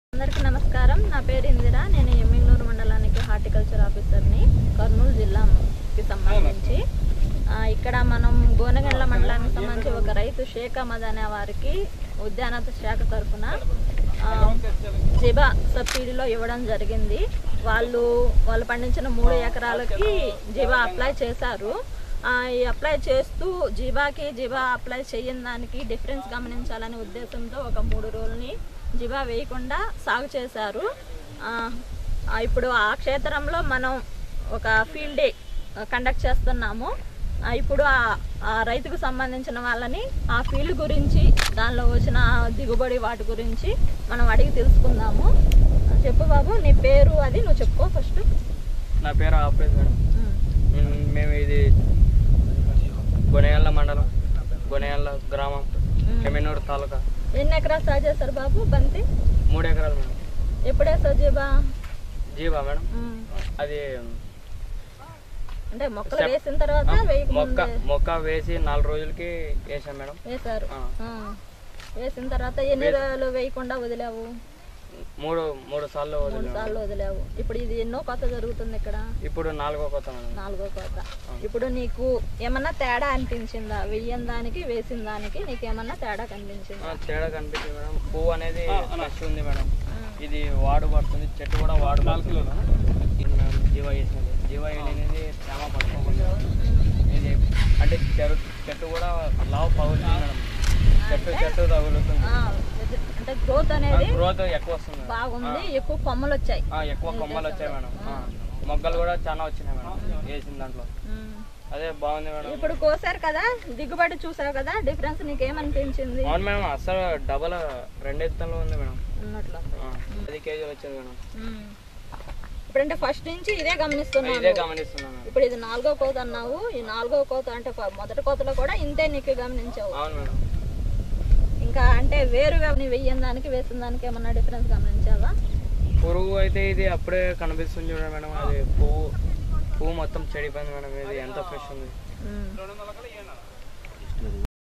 Zeba Chronicles - Marigold - Kurnool - Shaikh Ahmed
For Shaikh Ahmed, Zeba proved to be the missing ingredient for a bumper harvest from his marigold crop.